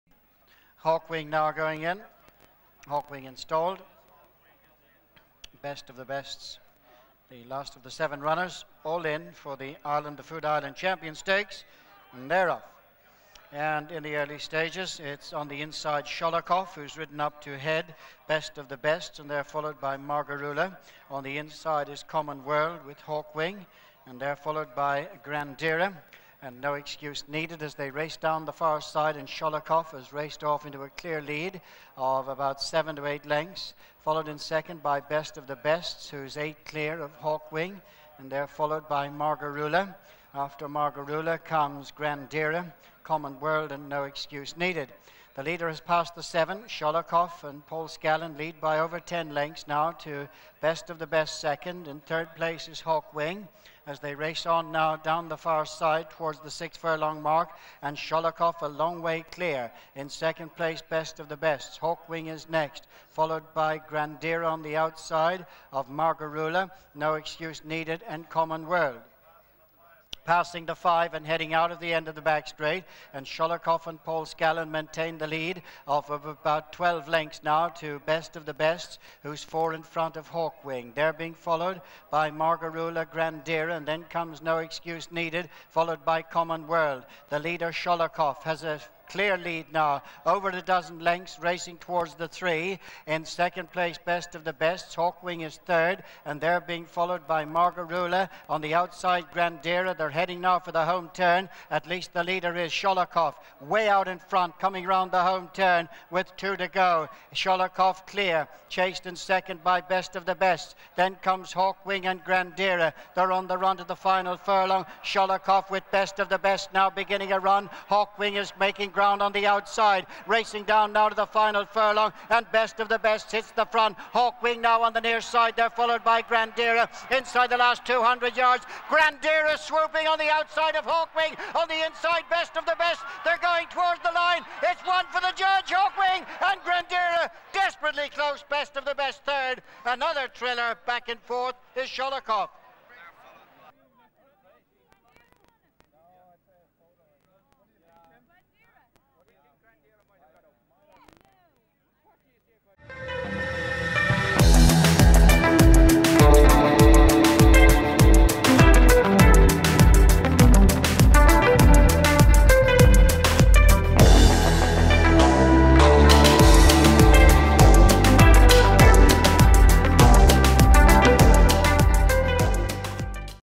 GRANDERA and Frankie Dettori edge out HAWK WING in the 2002 Irish Champion Stakes at Leopardstown
Remember the days when the pacemakers used to go off miles in front? A fascinating tactical battle here for the 2002 Irish Champion Stakes as Grandera denies Hawk Wing in the very last strides after Ballydoyle's Sholokhov set fearsome fractions from the front.

It was a fourth win in the last five years in this race for Godolphin.